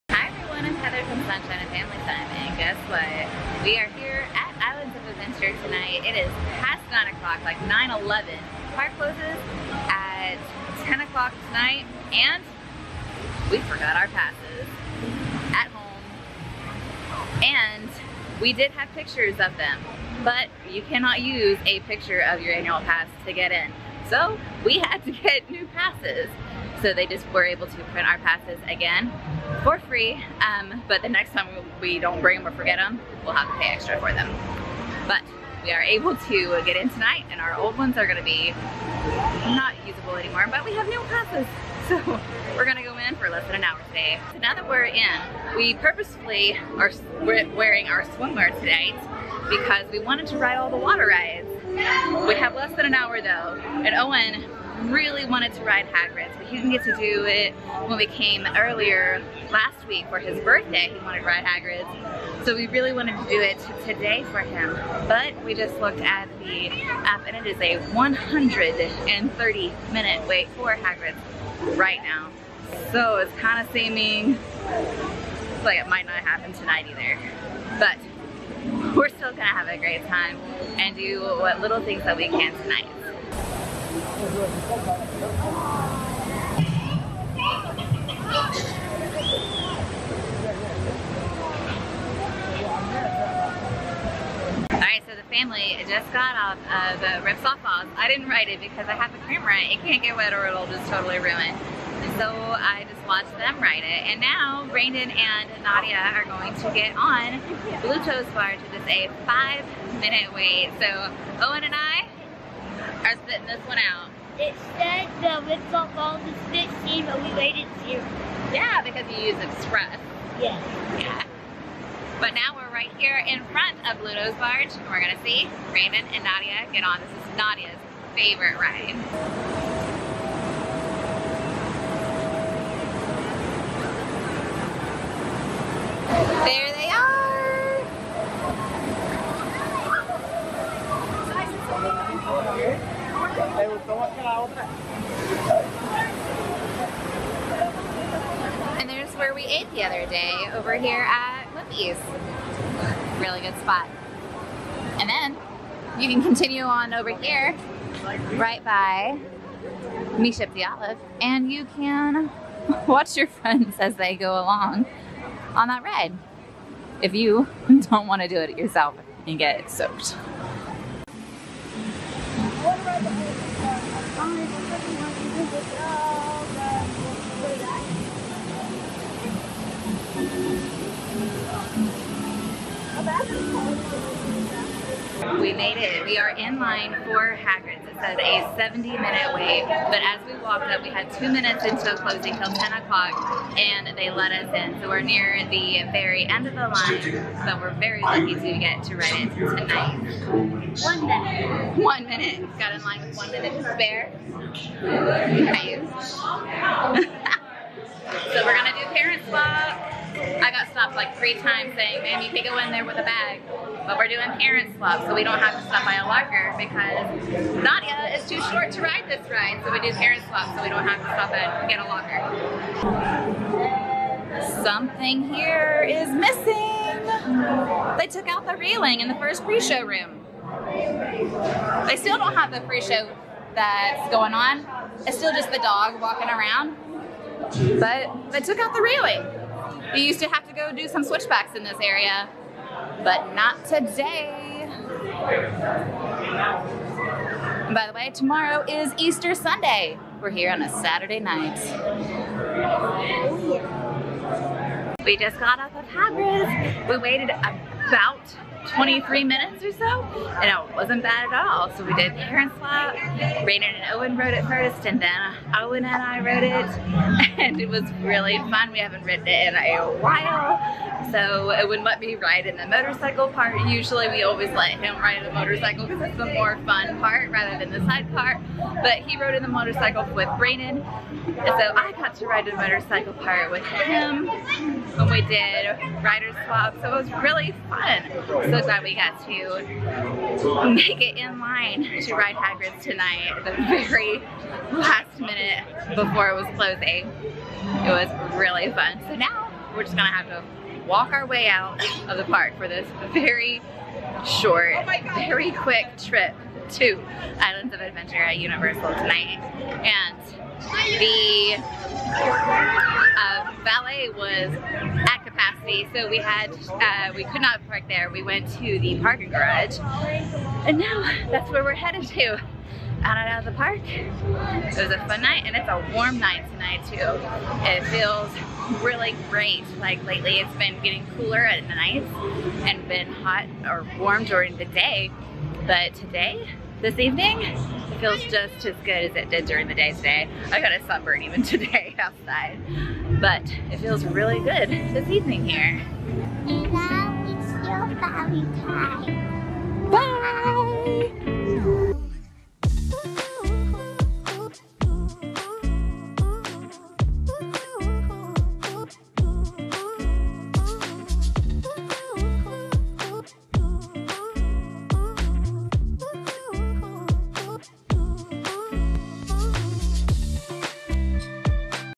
3 Rides In Under An Hour At Islands Of Adventure
Today we went to Islands of Adventure-and forgot our annual passes! We were able to get some new ones printed though! Once inside, Brandon and the kids rode Rip Saw Falls and got very wet! Then Brandon and Nadia rode Bluto's Barge while Owen and I watched from Me Ship the Olive. Surprisingly-we were able to get in line a minute before the park closed for the night for Hagrid's Motor Bike Adventure! The wait time said 80 minutes-but we only waited 23! We had a great time with our family!